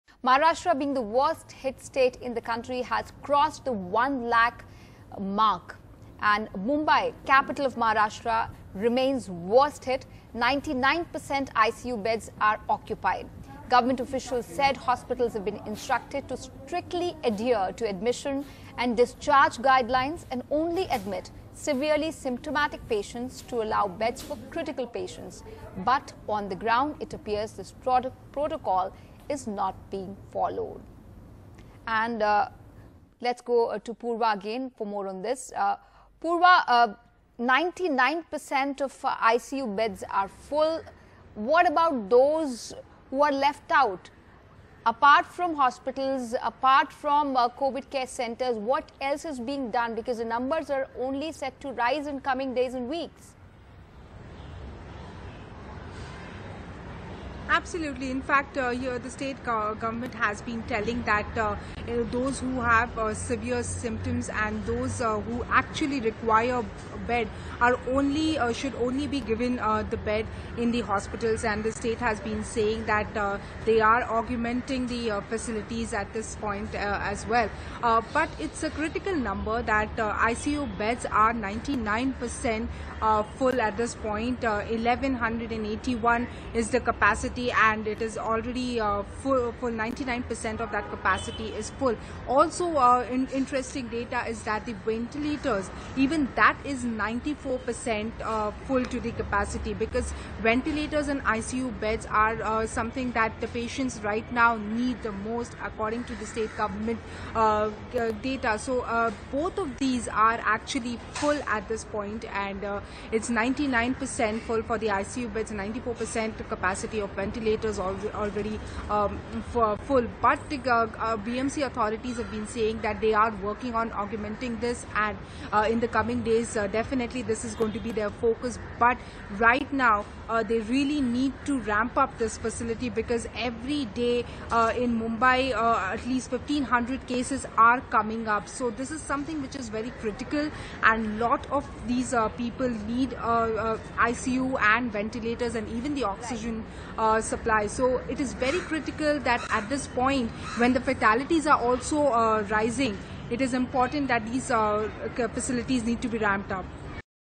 Covid-19 Update: 99% ICU Beds, 94% Ventilators In Use As COVID-19 Cases Spike In Mumbai
Intensive Care Units (ICUs) in Mumbai, the city worst-affected by the novel coronavirus outbreak, are operating at 99 per cent capacity and 94 per cent of ventilators in the city are currently in use, the Brihanmumbai Municipal Corporation (BMC) said on Saturday.